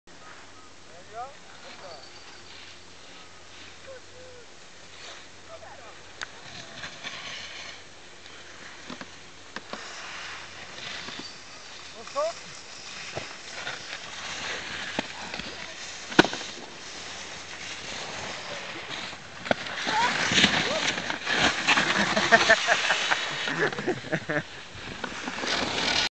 Rob Cragg takes on the half pipe - and it spits him out.AVI
New Year's Day 2011 and Skanky takes on the half pipe and loses . . .